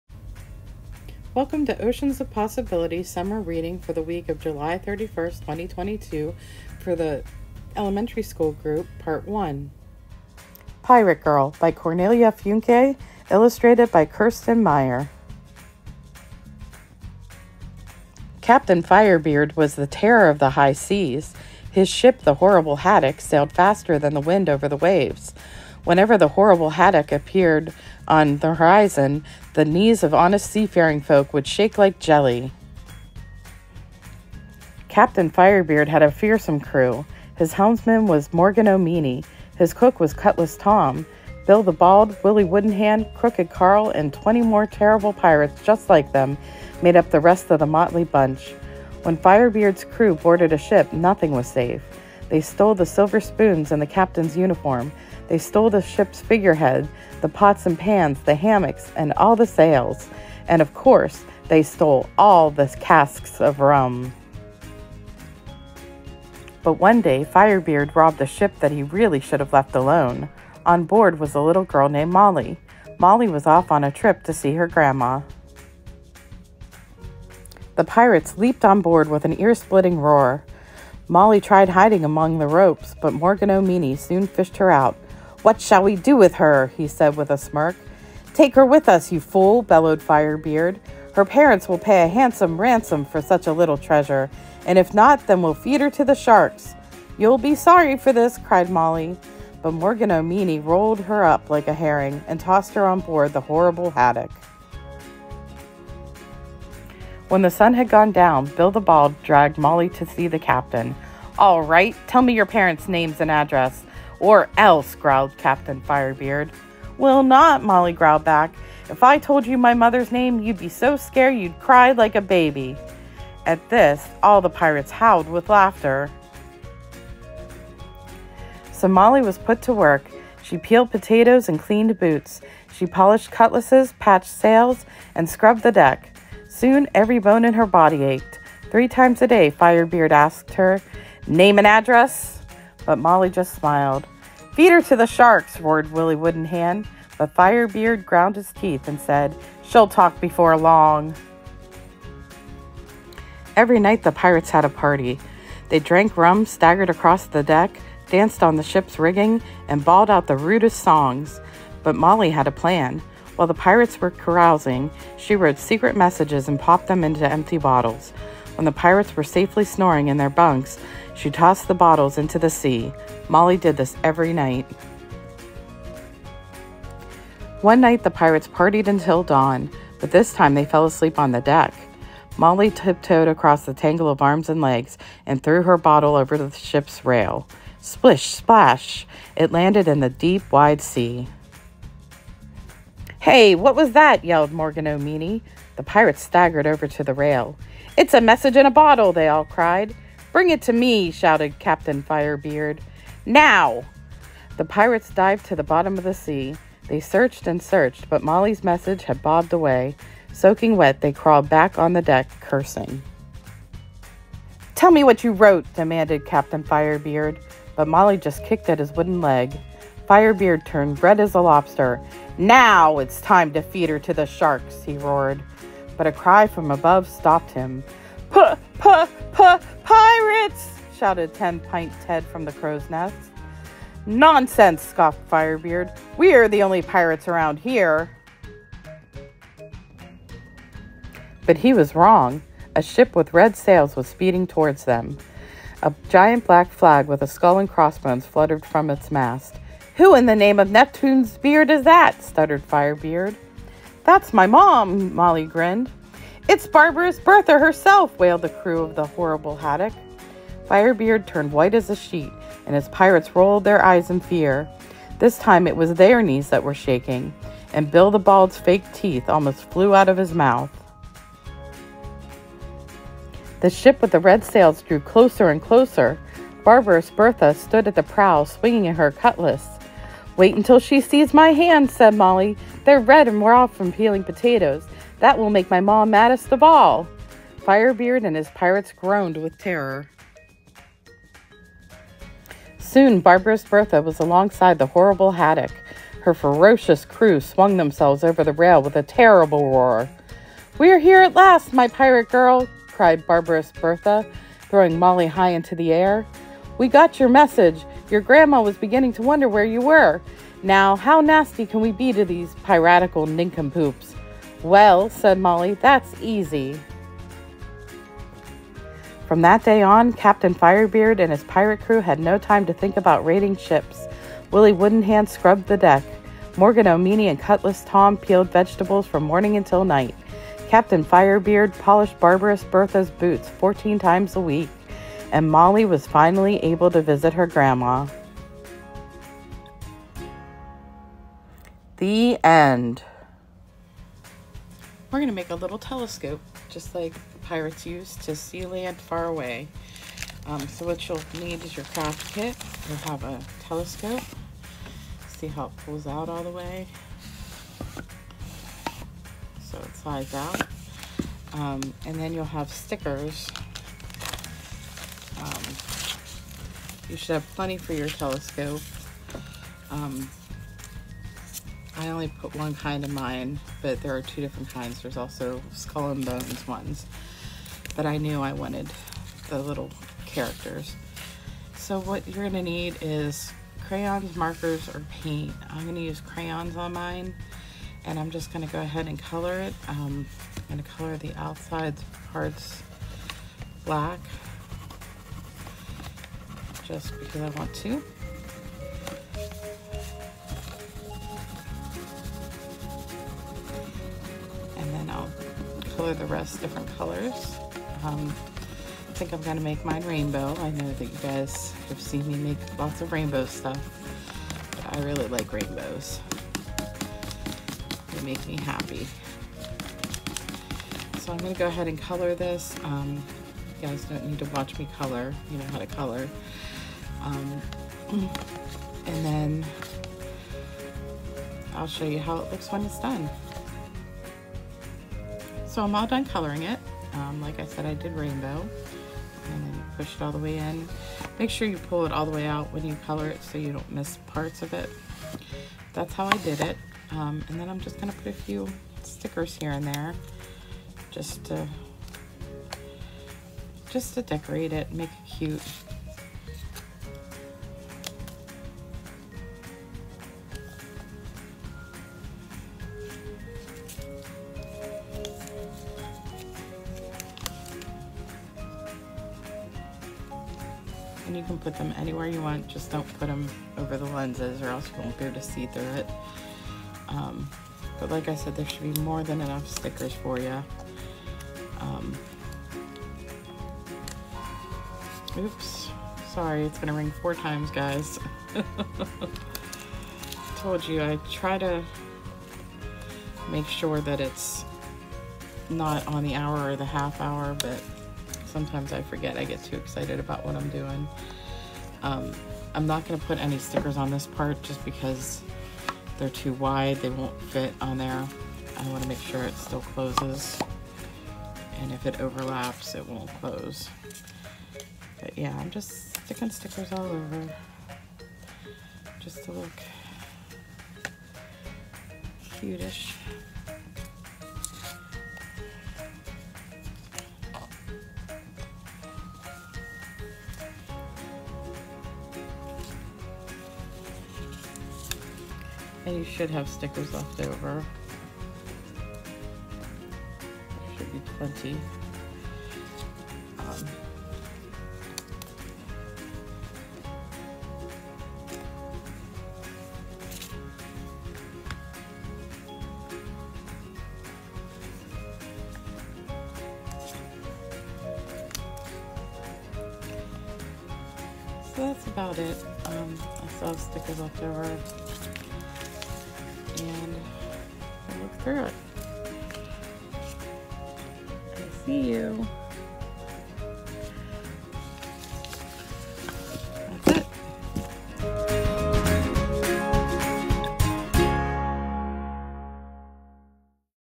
Oceans of Possibilities—Elementary—Pirates—Week of 7/31/22 Part 1
Oceans of Possibilities Summer Reading
Week of 7/31/22
Elementary Group Part 1
Theme: Pirates
Story: Pirate Girl (Cornelia Funke)
Craft: DIY Telescope
Craft to Go Kits available on the front table in the Children’s Area at Mary H. Weir Public Library 8/8-8/12/22.